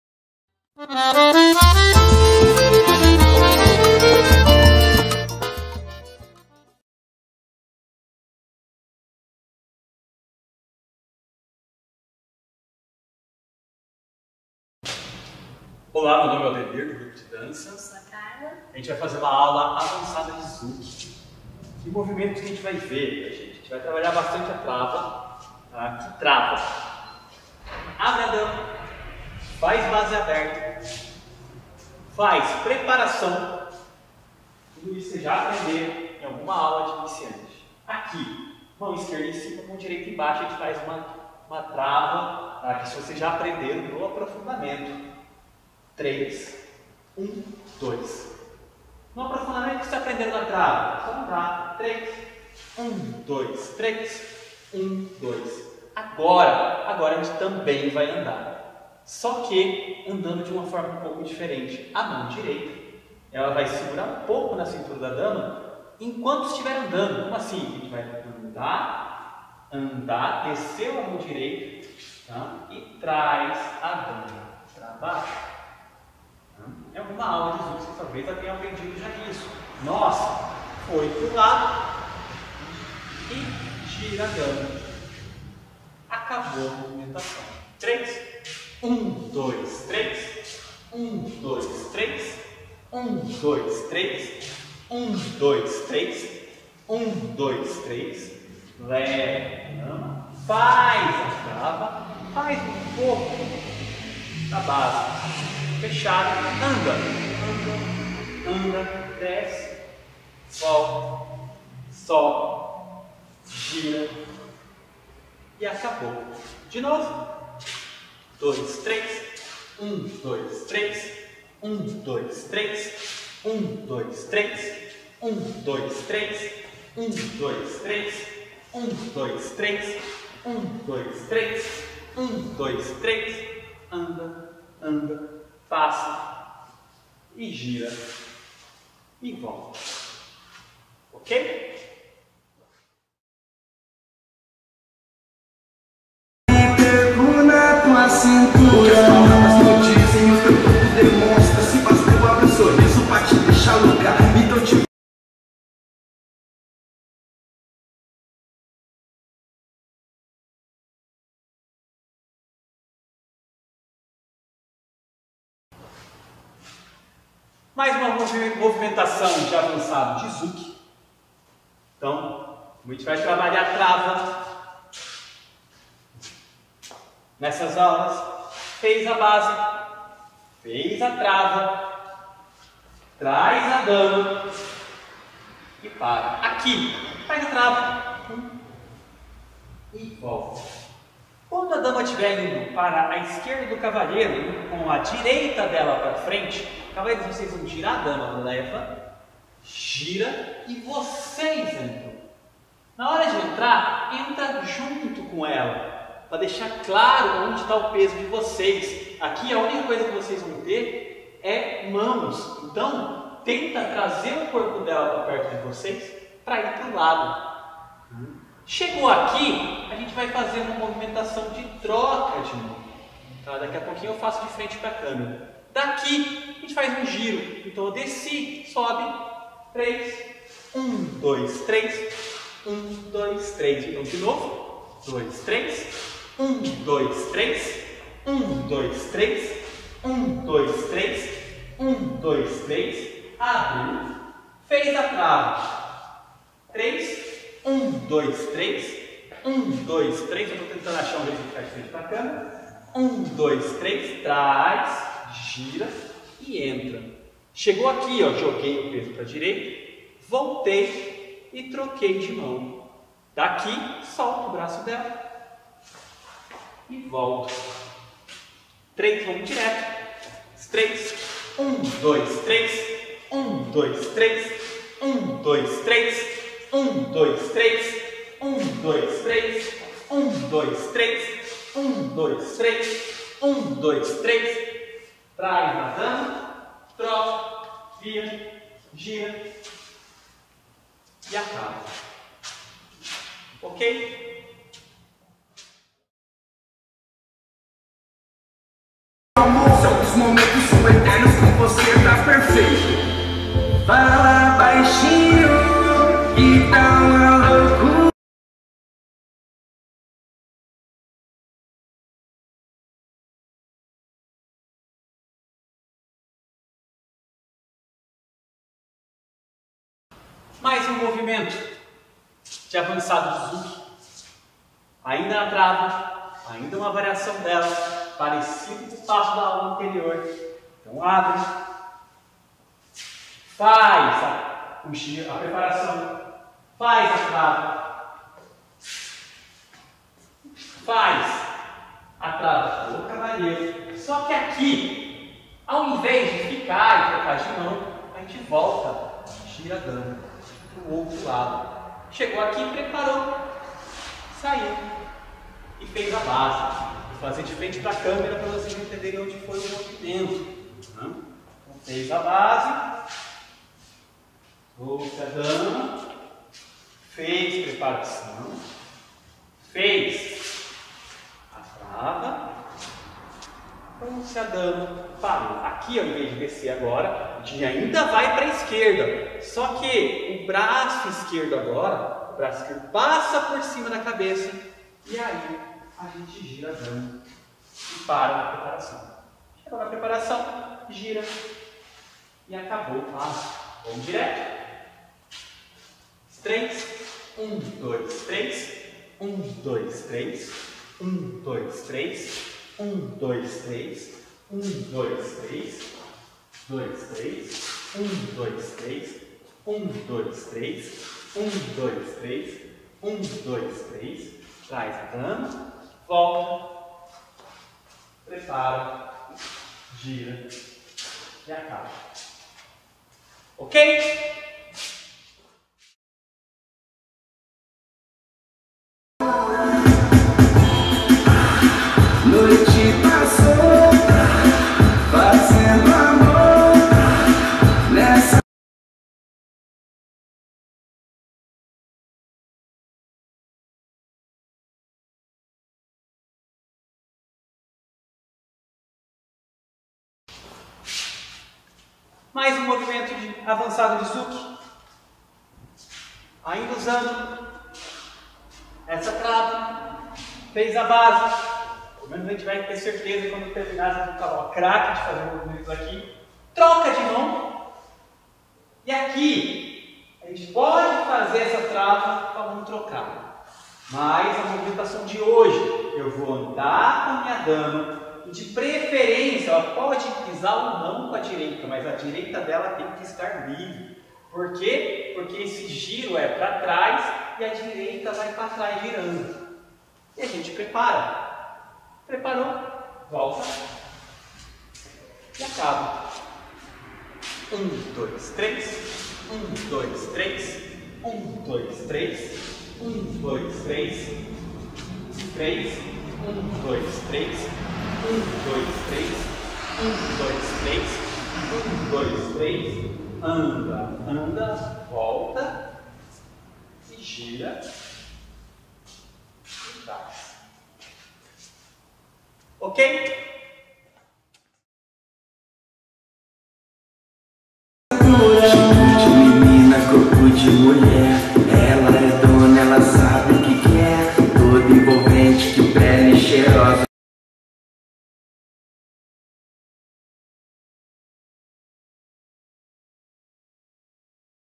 Aula Semanal - Zouk - Aula #011 - Curso 02 - Avançado - Parte 1
Mais um curso de Zouk para vocês. Avançado, com passos importante para se usar no salão

Gostou das aulas, nos apoie e garanta que teremos aulas toda semana ou todos os dias para sempre :D